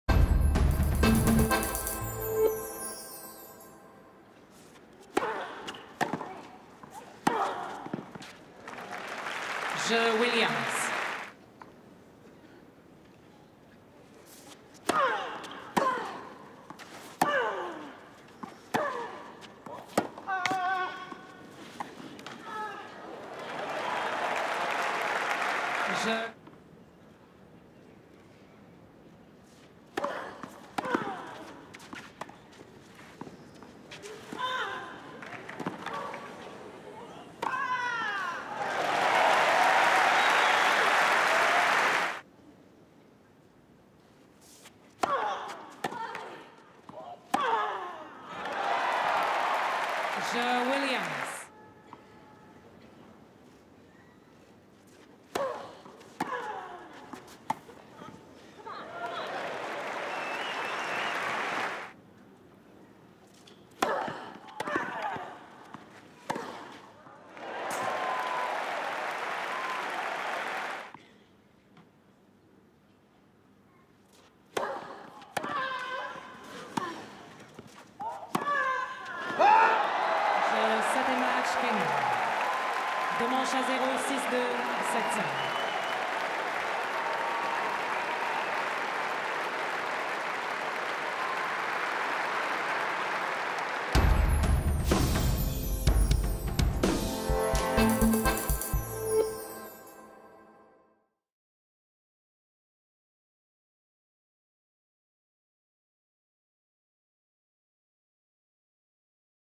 Serena Williams vs Sofia Kenin - Round 3 Highlights | Roland-Garros 2019
Serena Williams vs Sofia Kenin - Round 3 Highlights | Roland-Garros 2019. Watch the best moments from the match that opposed Serena Williams and Sofia Kenin at the third round of RolandGarros. Sofia Kenin goes to round 4 with 6/2; 7/5.